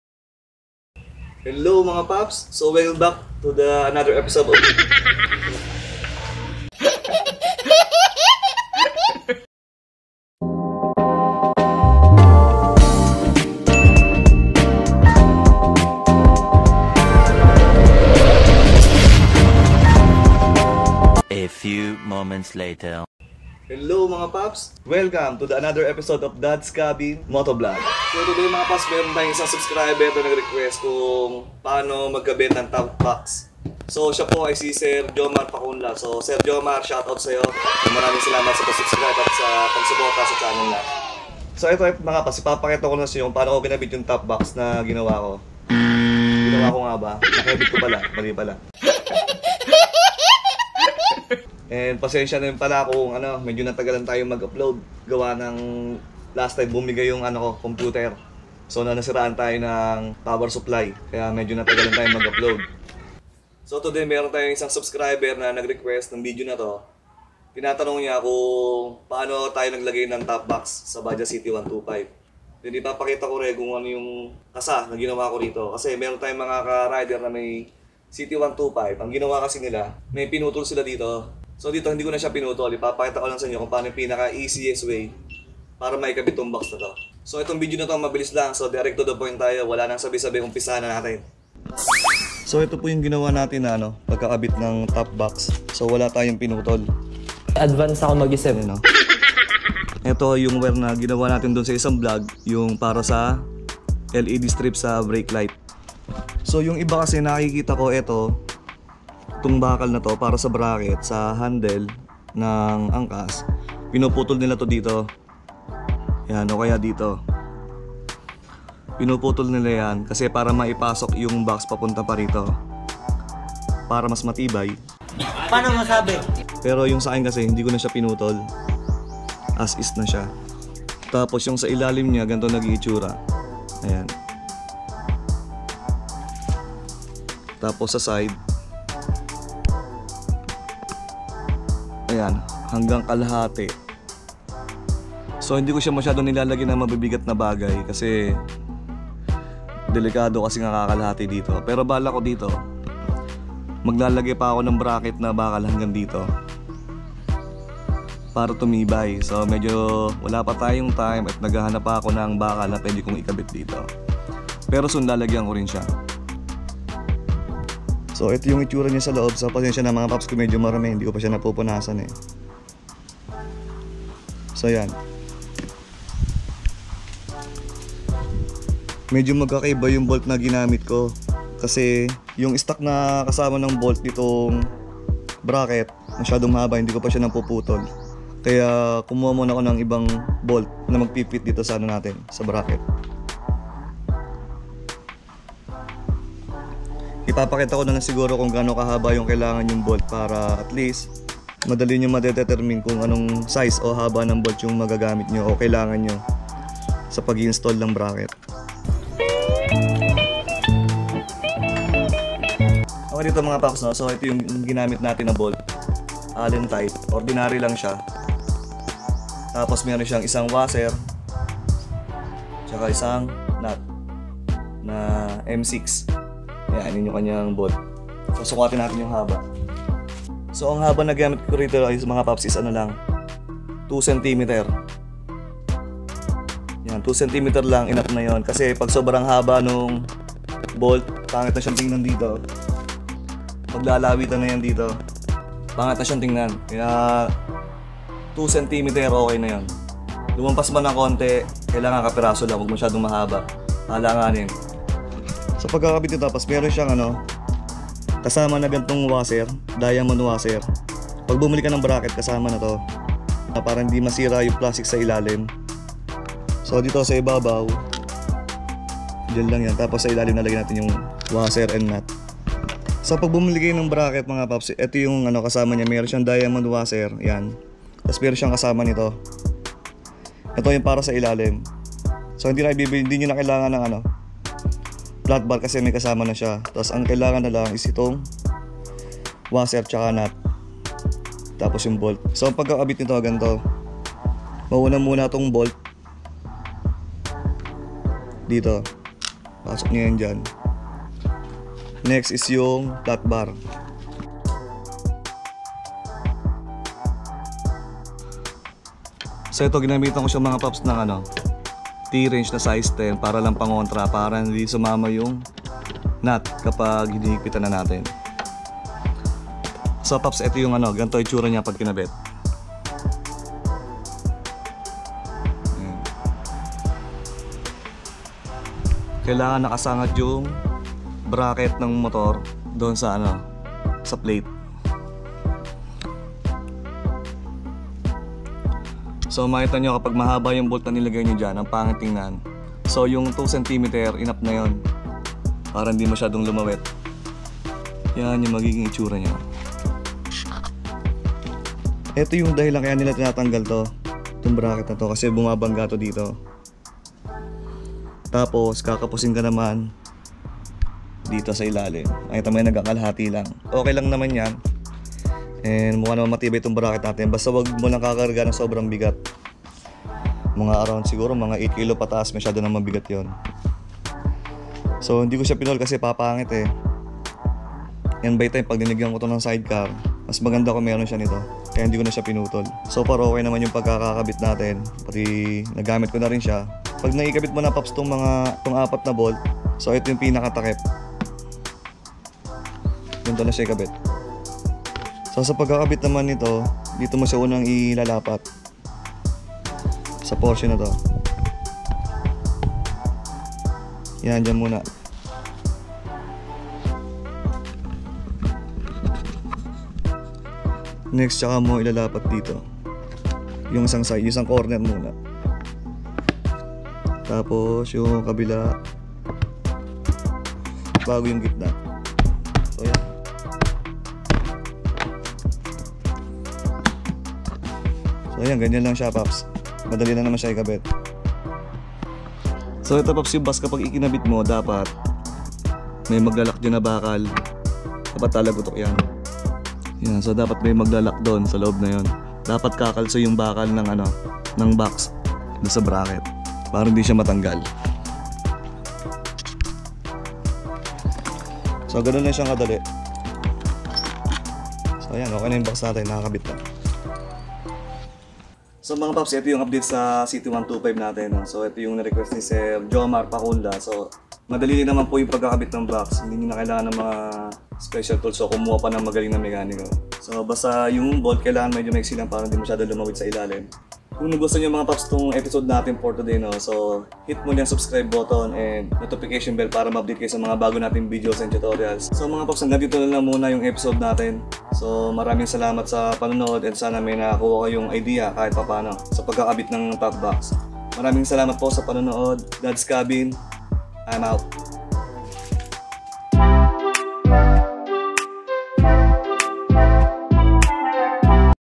PAANO MAG INSTALL NG TOP BOX/GIVI BOX
Share ko po mga paps kung paano ko ikinabit yung top sa bajaj ct125 ko...